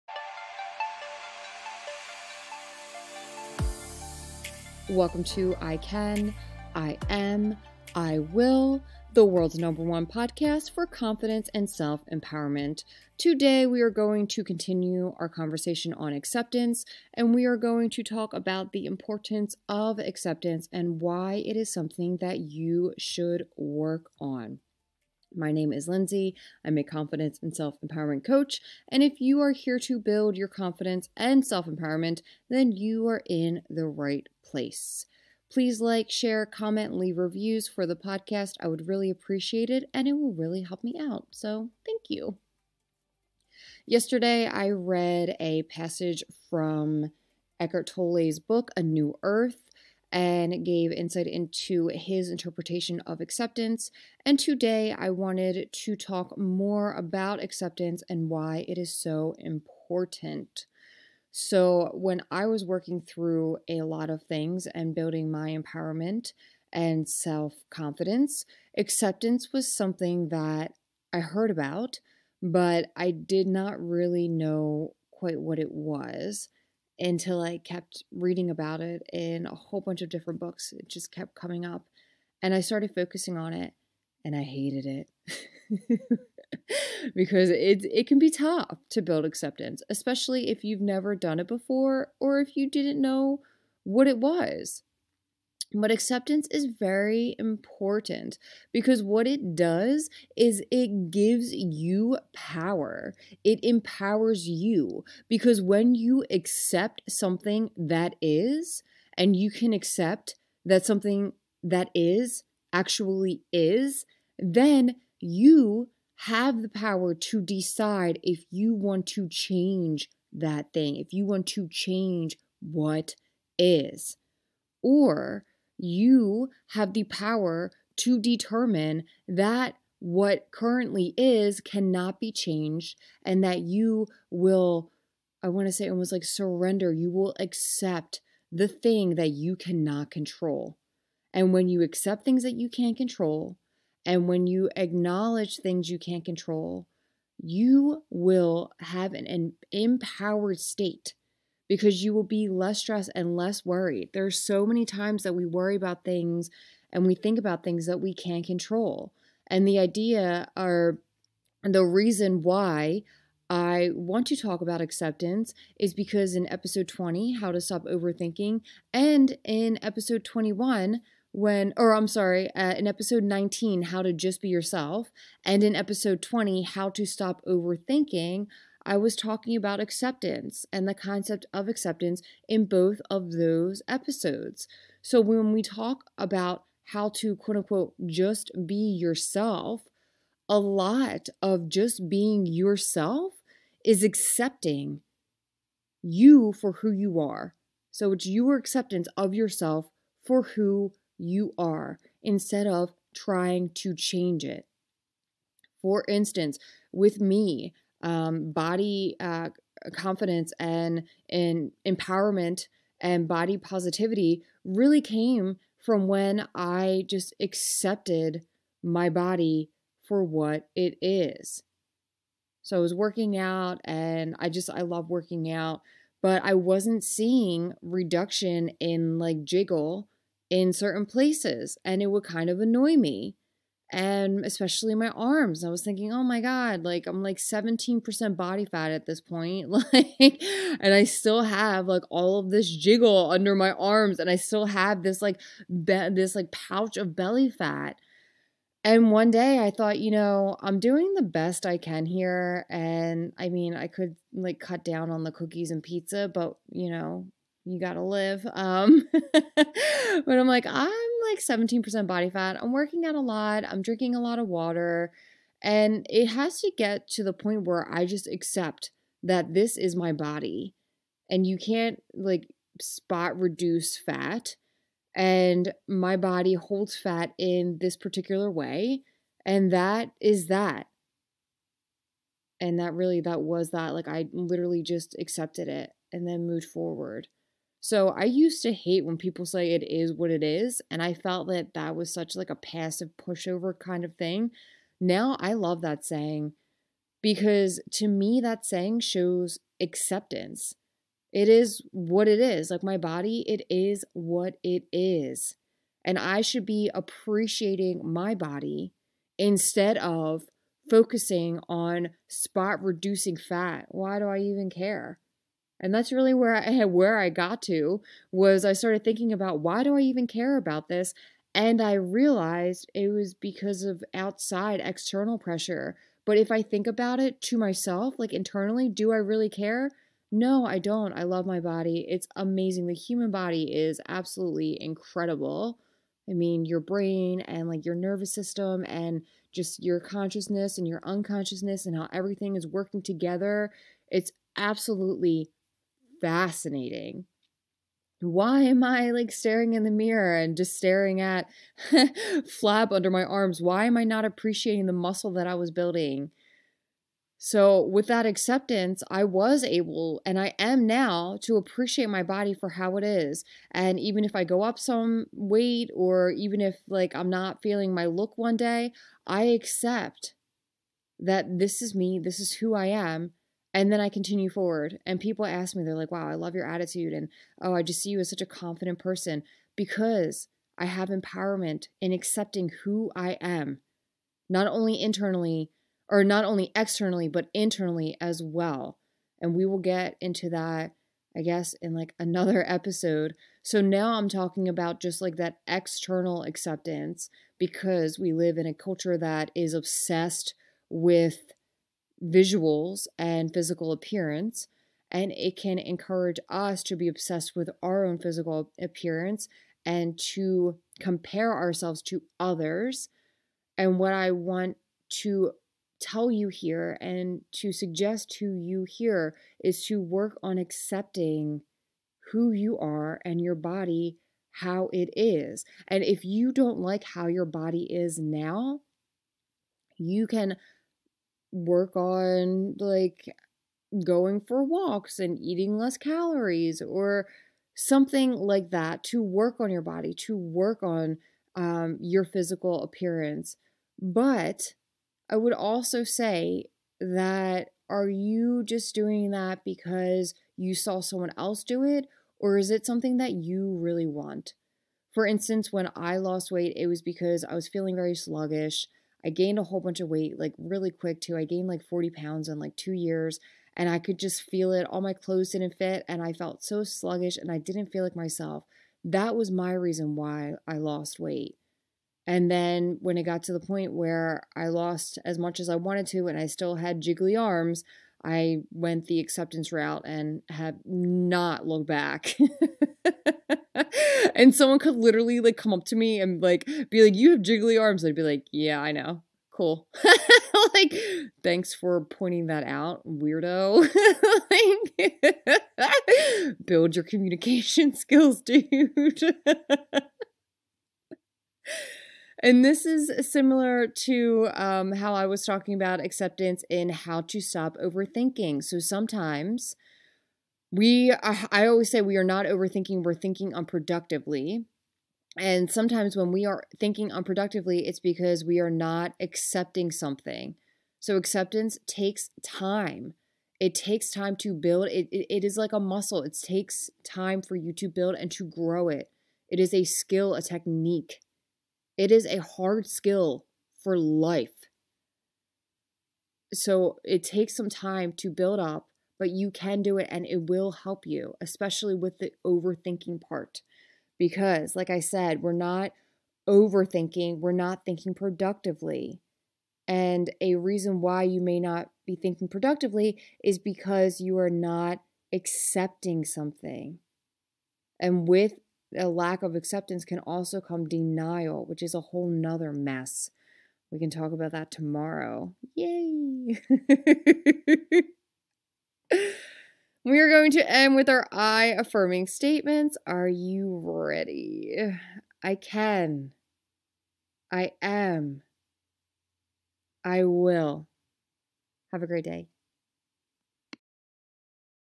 Acceptance, Empowerment & Body Positivity
In this episode we talk about how to use acceptance to feel empowered, build body positivity, get body confident and stop overthinking.